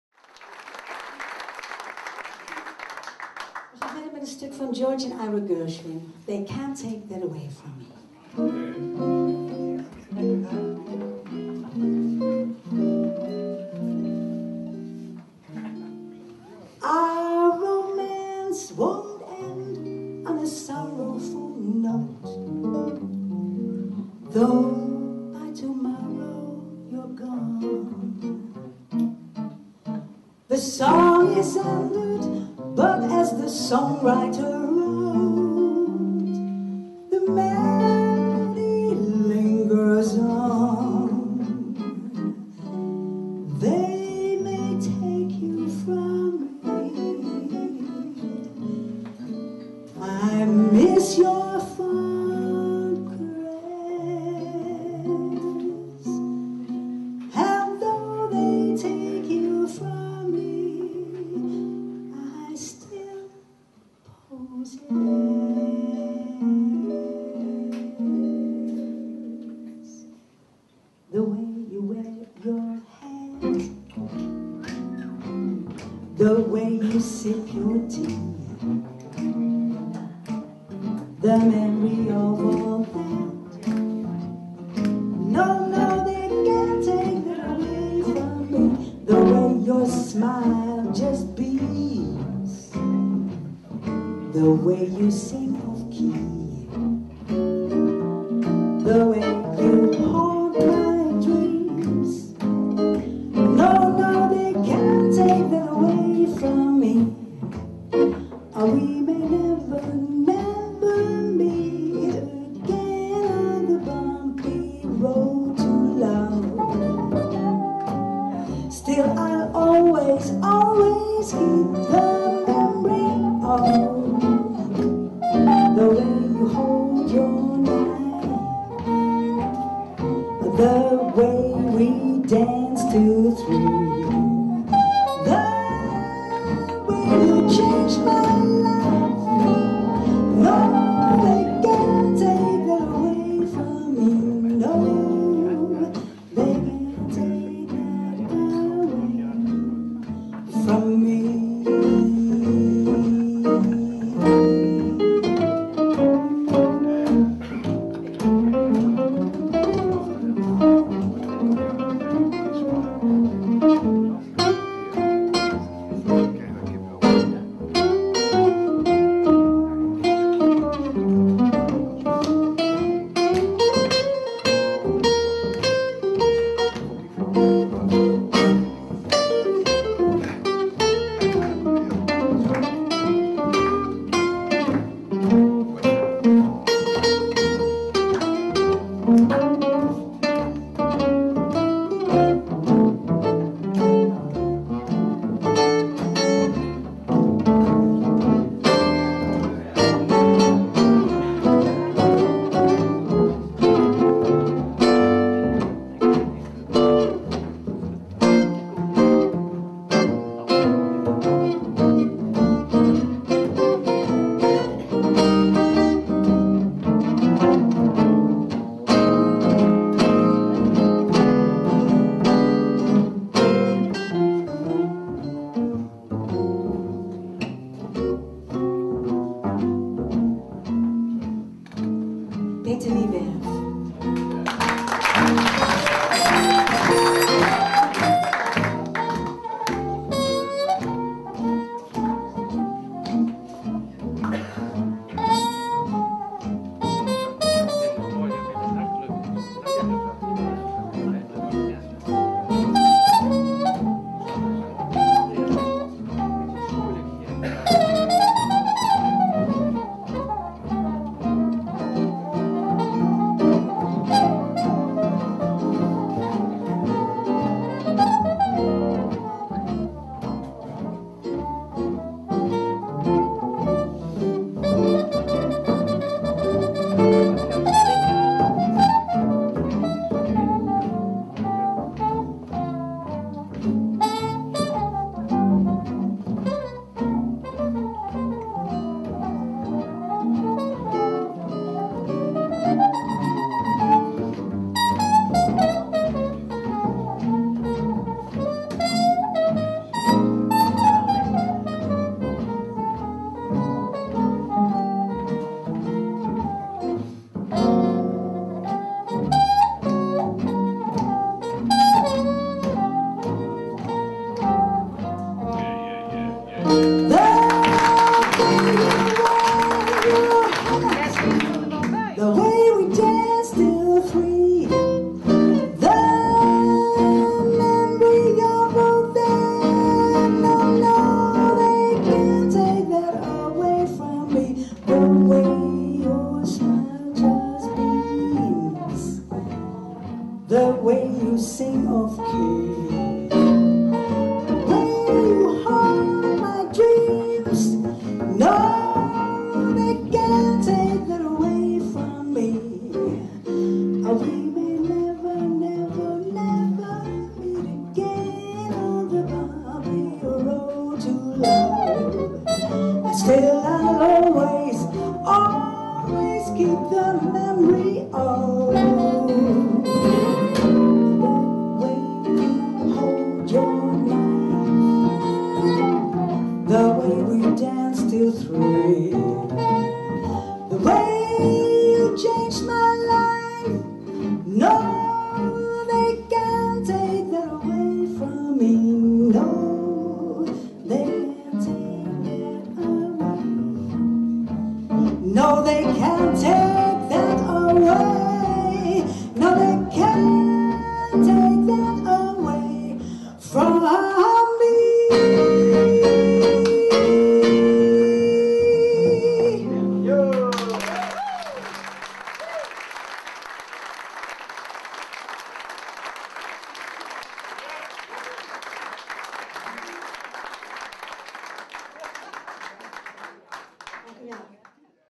Greetje Kauffeld & trio Ruud Ouwehand 2008
Greetje Kauffeld & trio Ruud Ouwehand in Paradox tijdens het stranger than paranoia festival 2008